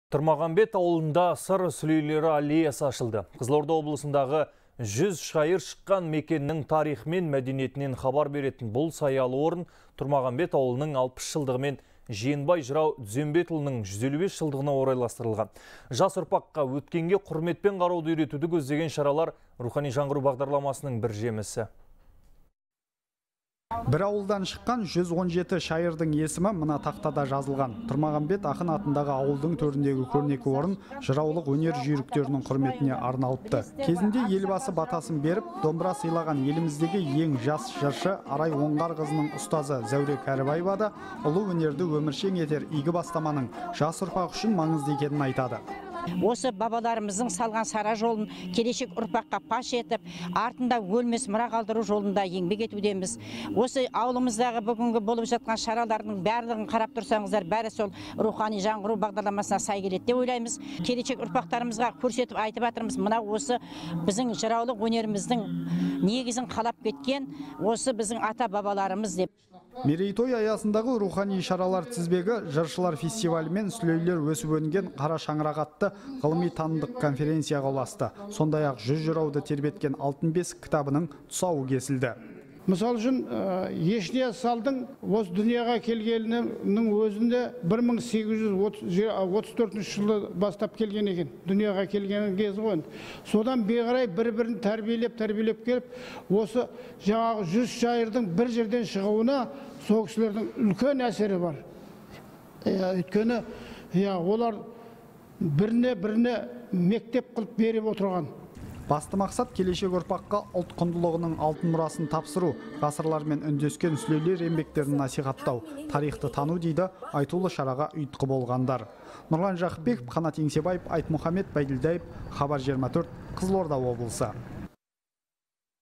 Қызылорда облысында Тұрмағамбет ауылының 60 жылдығы атап өтілді
Тұрмағамбет ауылында «Сыр сүлейлері» аллеясы ашылды, деп хабарлайды «Хабар 24».

Қызылорда облысындағы 100 шайыр шыққан мекеннің тарихы мен мәдениетінен хабар беретін бұл саялы орын Тұрмағамбет ауылының 60 жылдығы мен Жиенбай жырау Дүзенбетұлының 155 жылдығына орайластырылған. Жас ұрпаққа өткенге құрметпен қарауды үйретуді көздеген шаралар «Рухани жаңғыру» бағдарламасының бір жемісі.